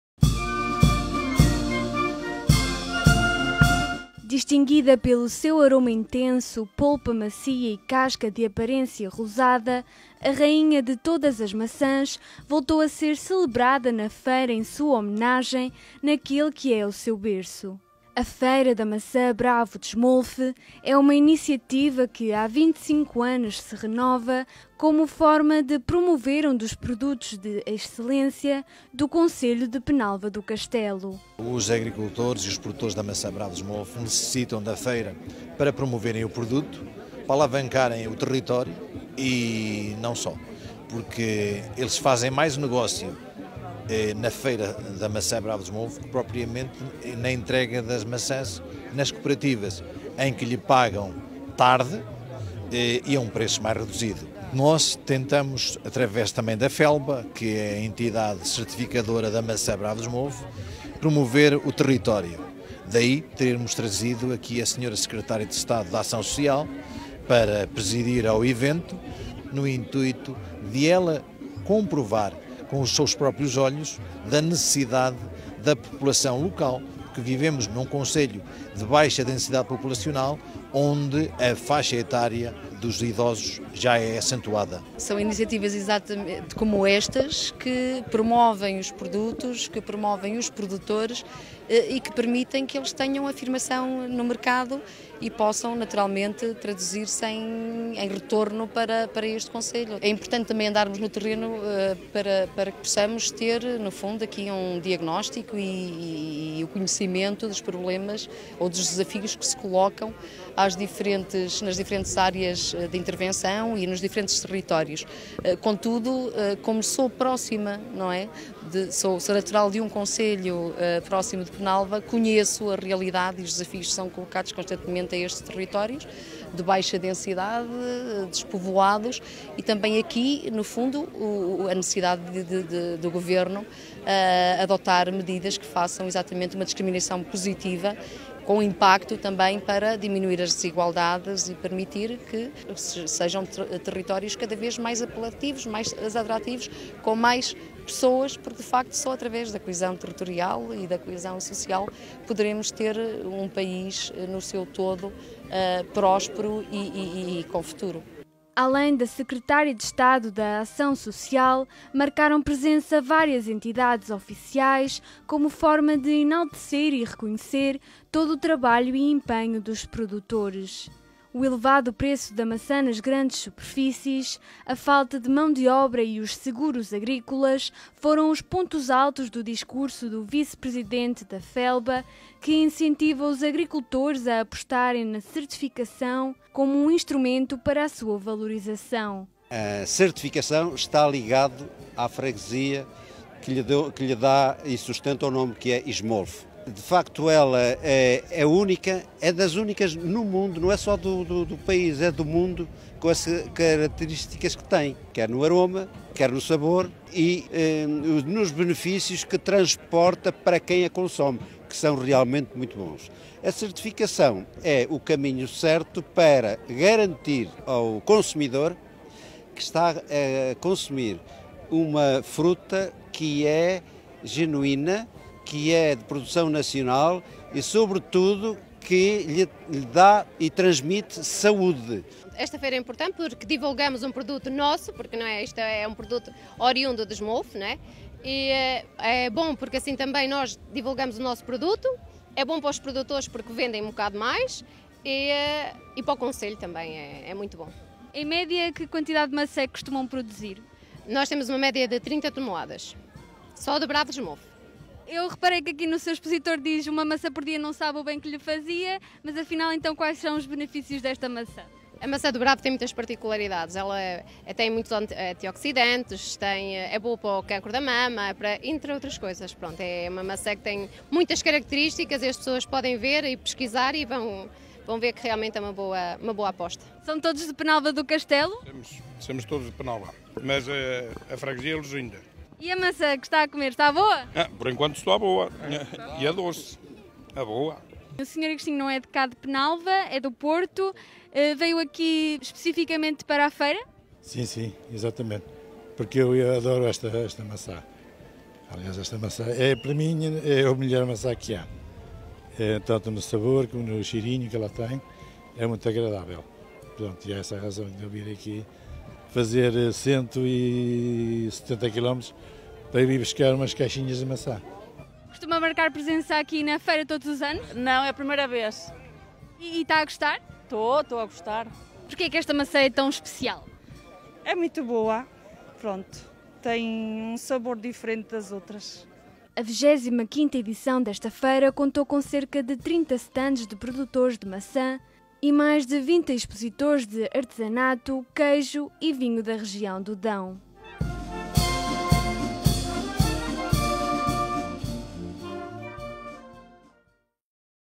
XXV Feira da Maça Bravo de Esmolfe
O Município de Penalva do Castelo voltou a celebrar a Maça Bravo de Esmolfe, um dos ex-líbris da fruticultura nacional, numa feira que vai já na 25ª edição.